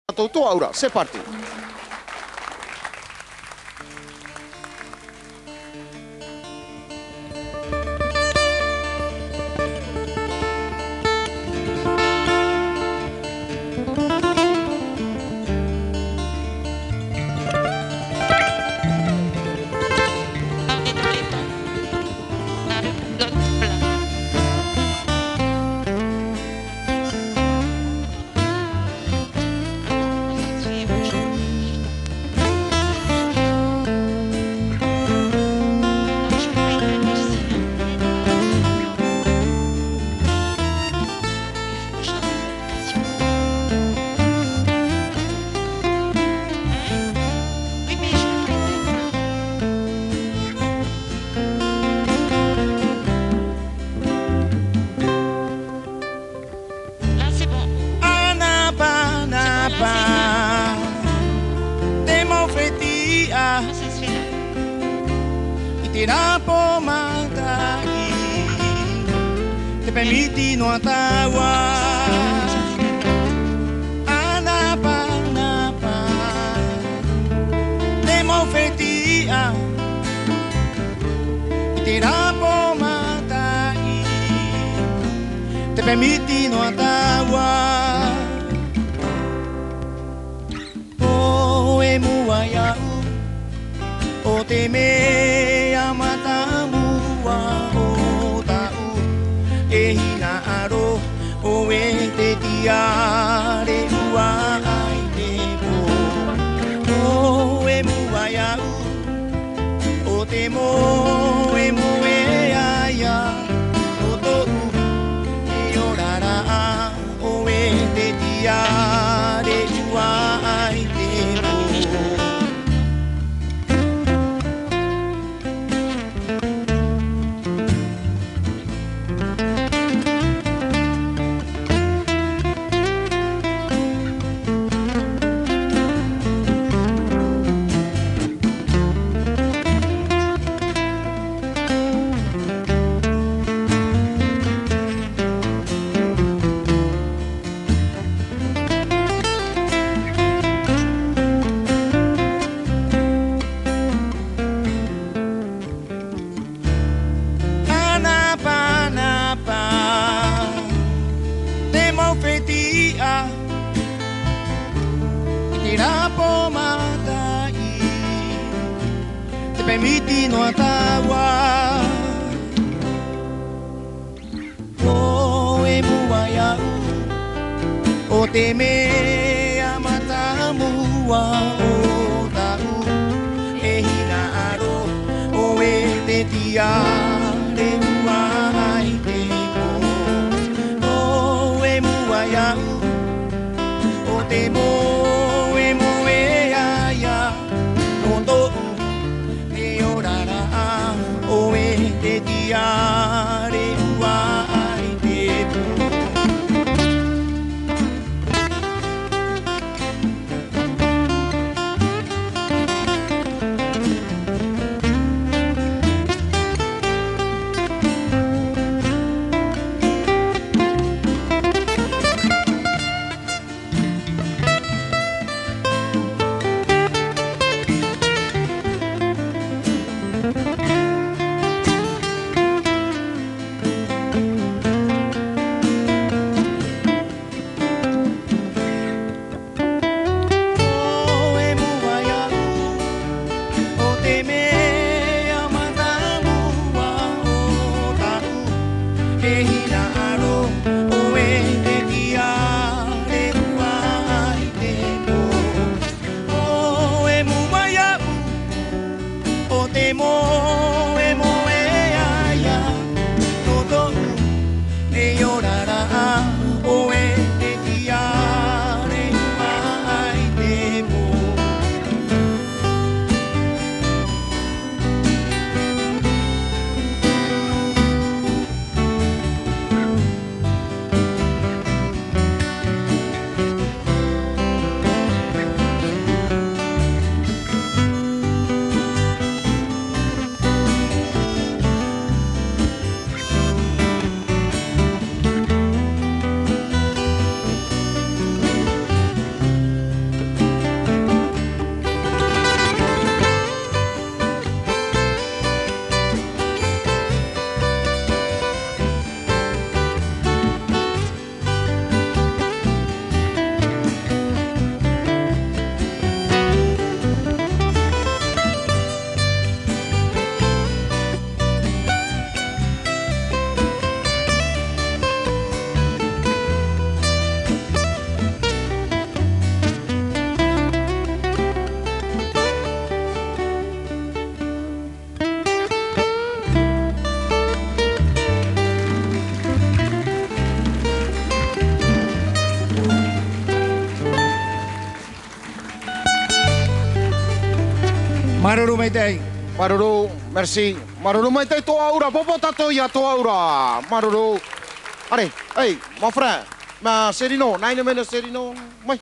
To'a Ura Part 1.
To'a Ura , un genre bien particulier de Polynésie ...